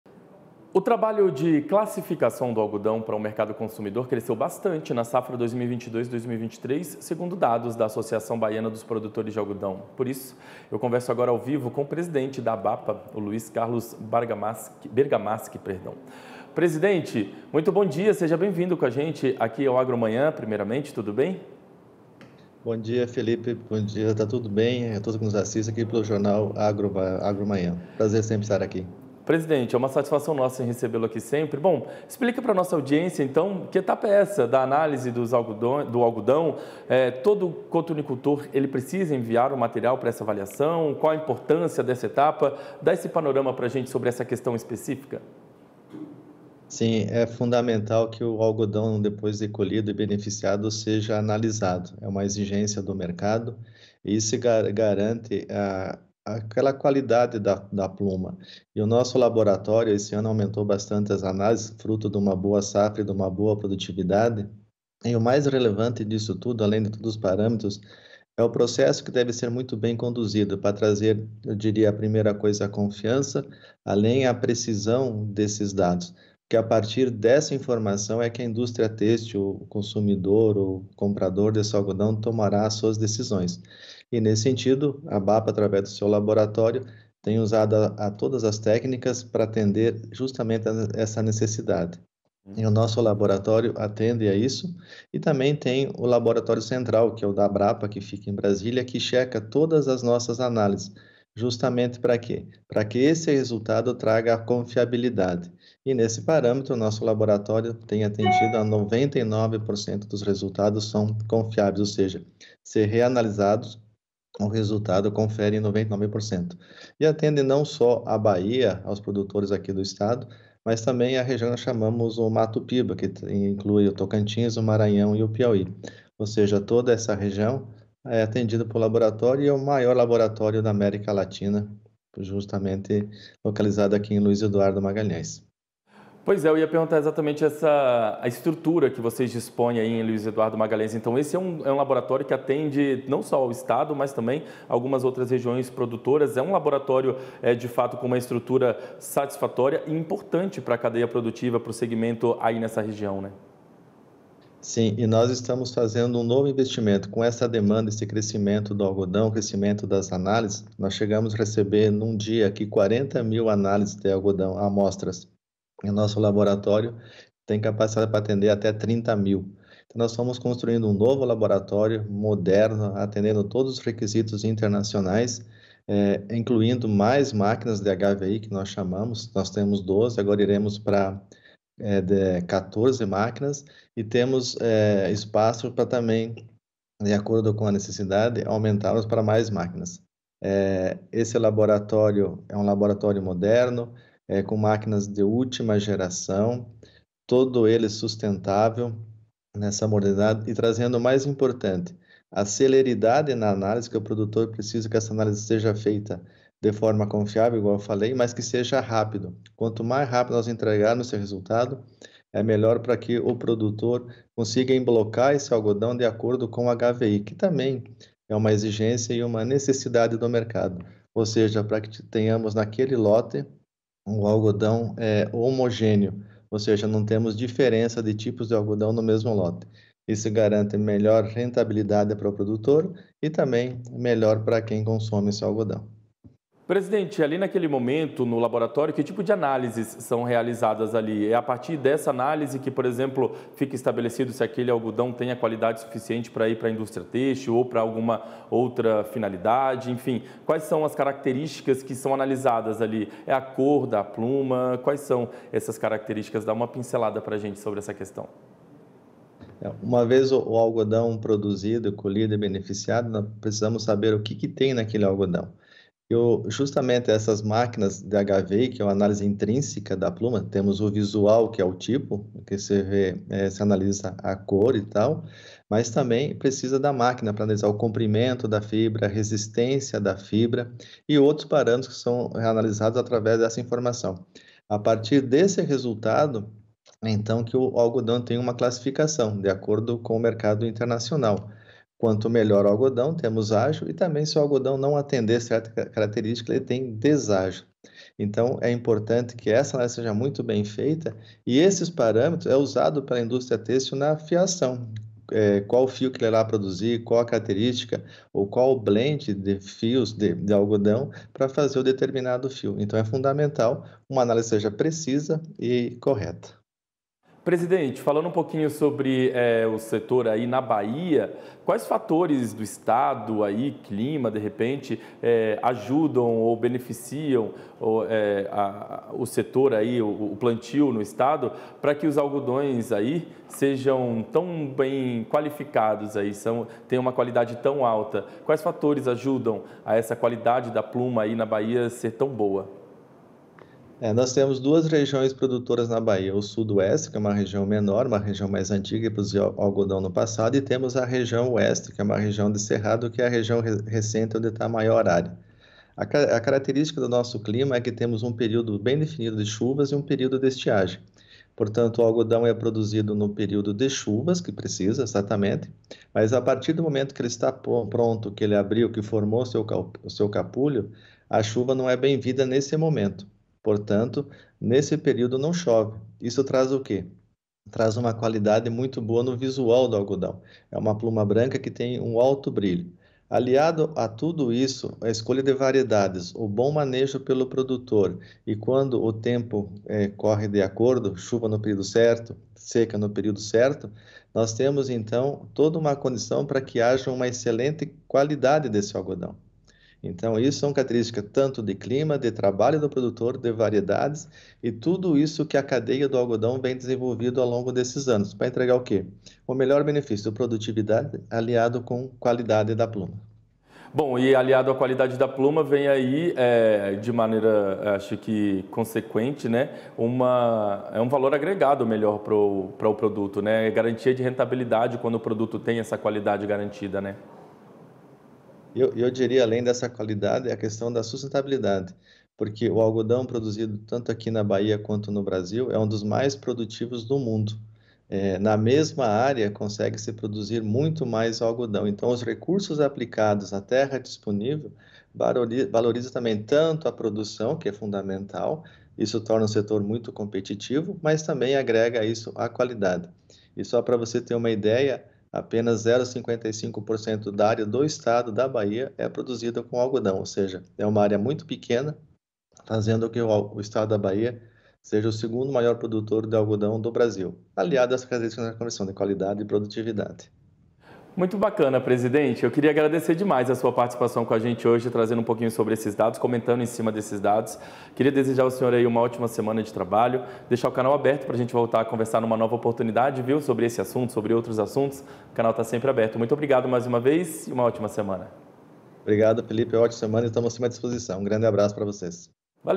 Bahia cresce em 25,75% as análises de algodão para o mercado
O trabalho de classificação do algodão para o mercado consumidor cresceu bastante na safra 2022/2023, segundo dados da Associação Baiana dos Produtores de Algodão. O presidente da Abapa, Luiz Carlos Bergamaschi, explica esse cenário promissor.